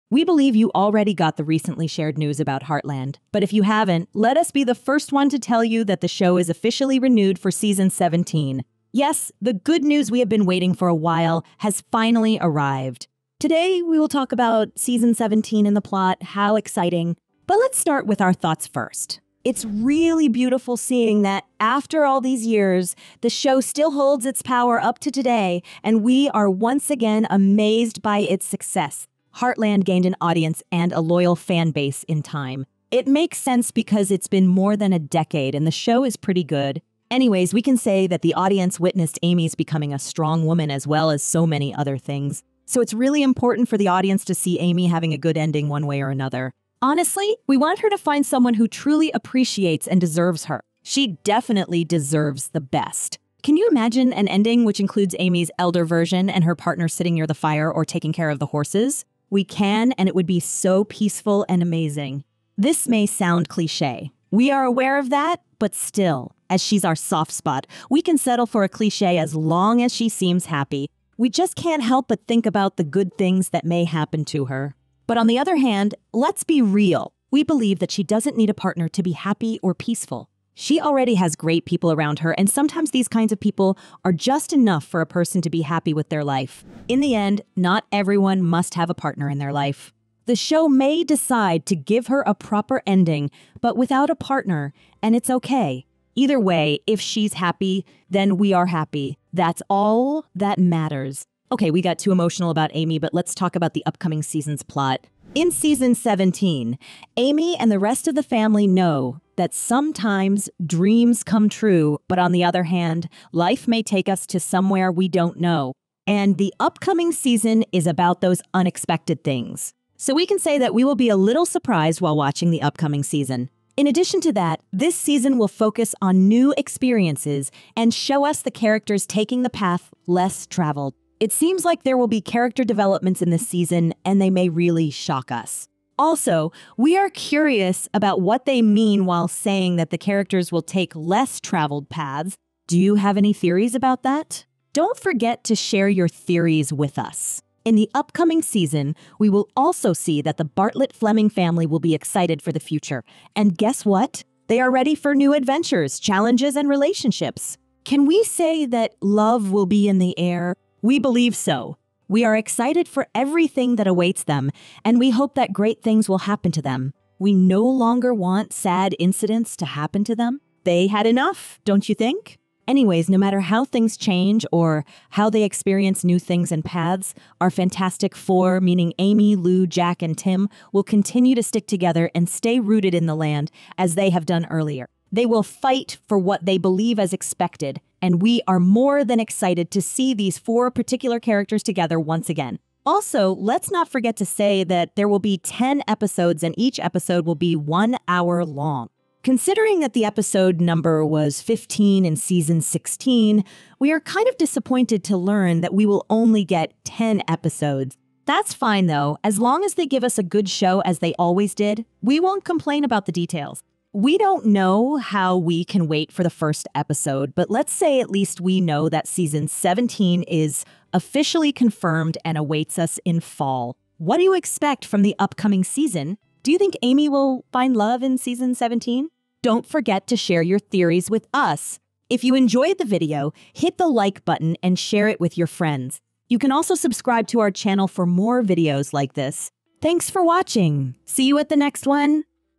Heartland Season 17 Plot Will Change EVERYTHING!
Since we know that Season 17 is on the way, let's talk about the plot! There are lots of adventures that awaits Bartlett-Fleming family, and we are down for it!